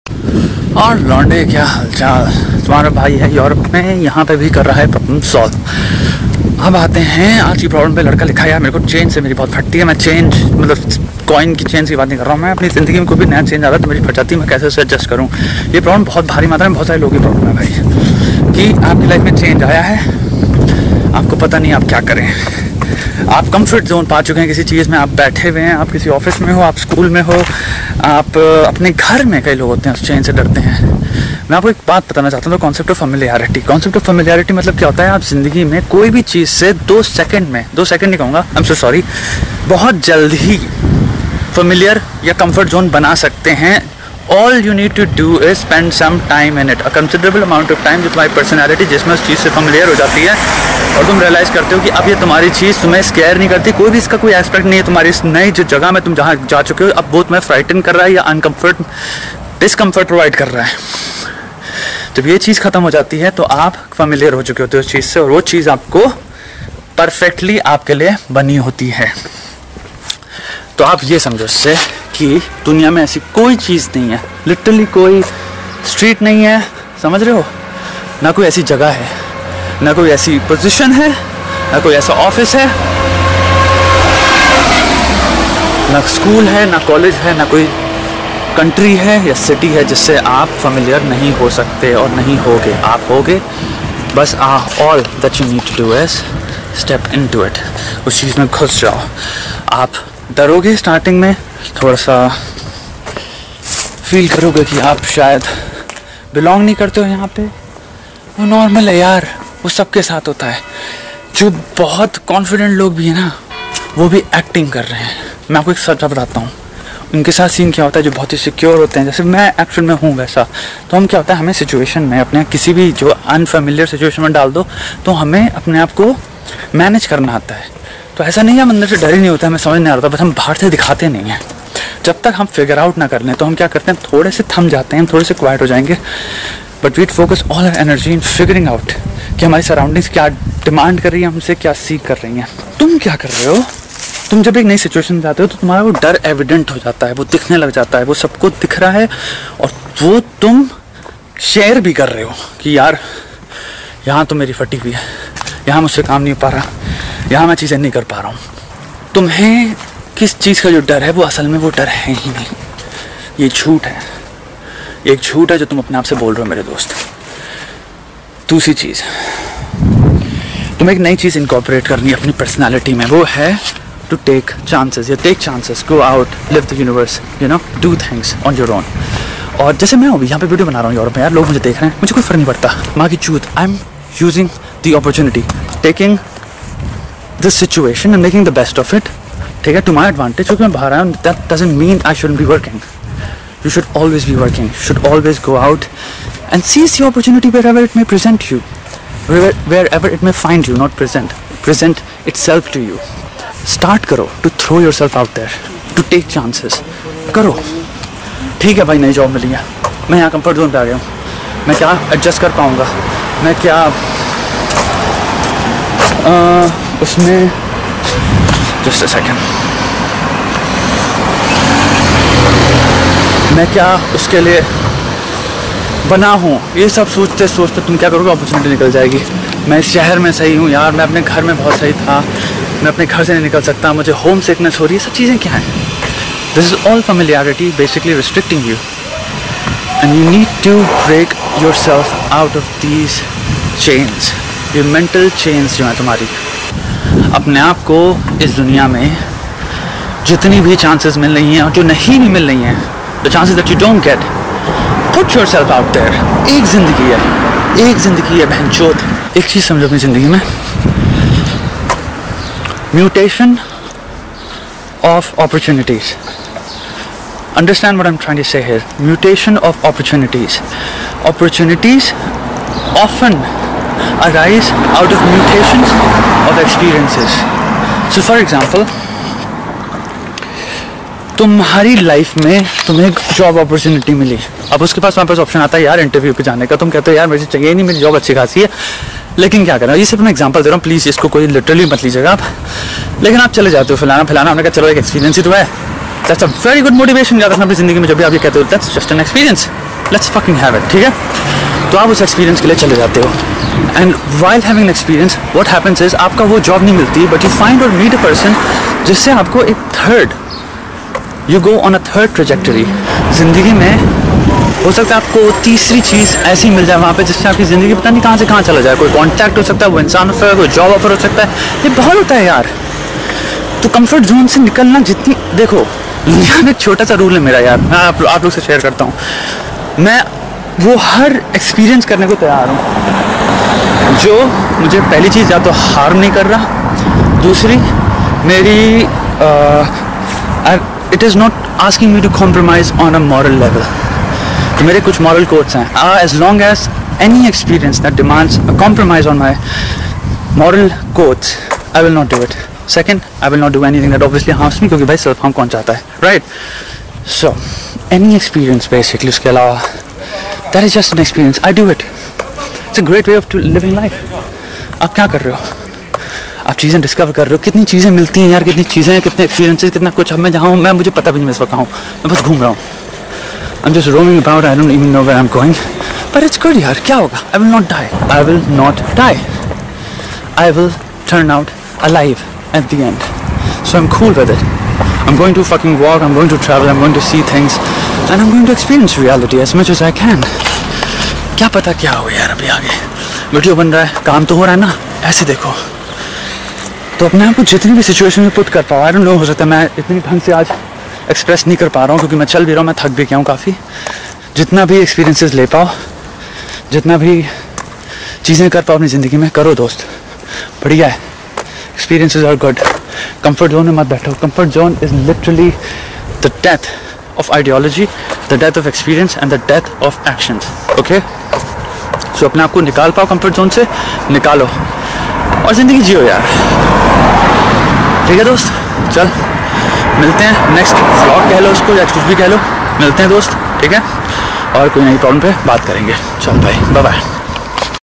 Are you stuck in Comfort Zone? (Europe Vlog)
Are you stuck in Comfort Zone? Are you afraid of change?

Message me there if you have a problem, and I will make a video on it.